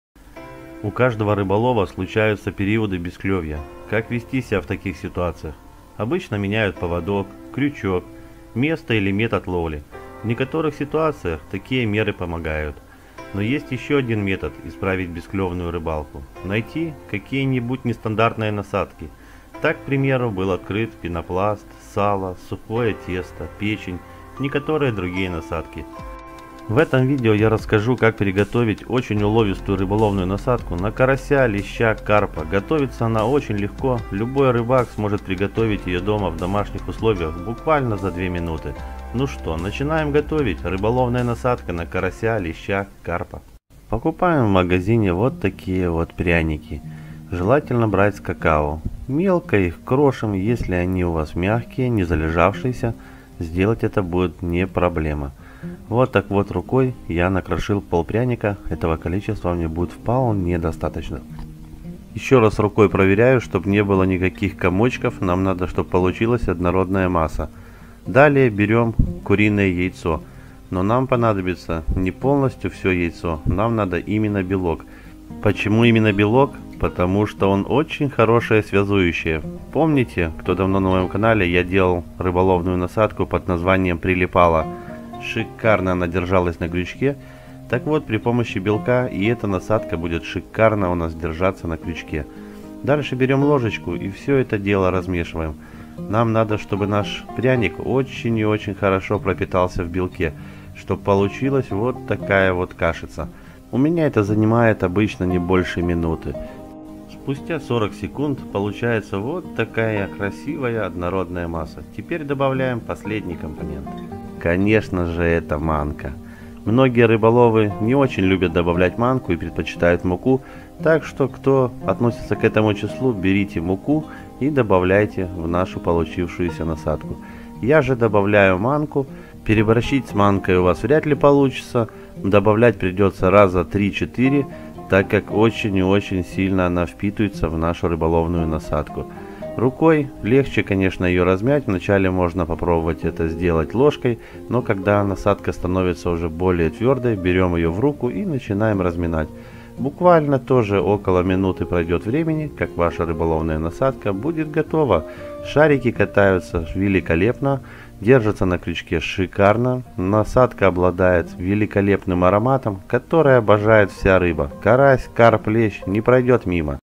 Супер клёв в мае! Насадка на Карася Карп Сазан Супер Рыболовная насадка на карася
Уловистая насадка для карася, карп, сазан и другая белая рыба.  Как легко приготовит насадку для рыбалки на карася карпа леща и не только. Такая насадка для рыбалки  отлично себя зарекомендовала среди рыбаков.